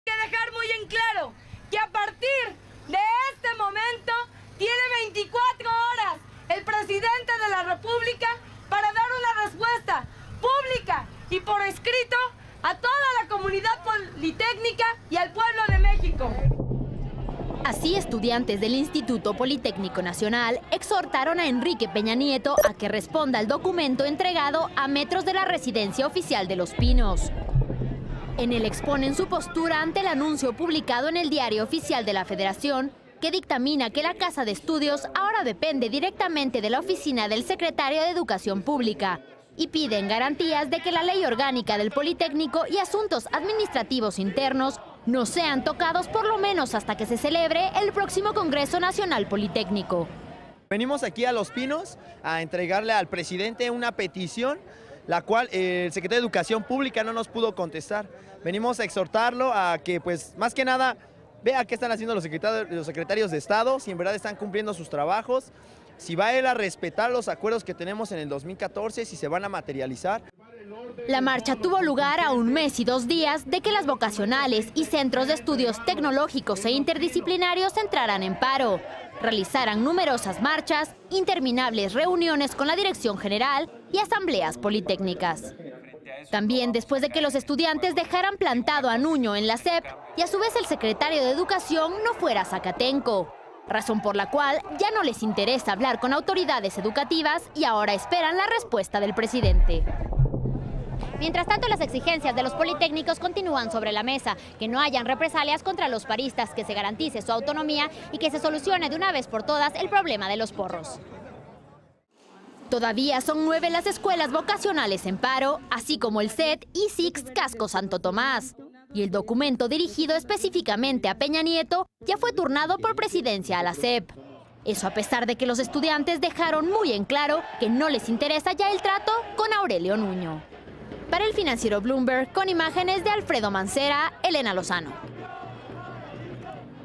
Estudiantes del IPN exigieron respuesta a Peña
Estudiantes del IPN marcharon a Los Pinos pedirle al presidente Peña Nieto que intervenga en el conflicto.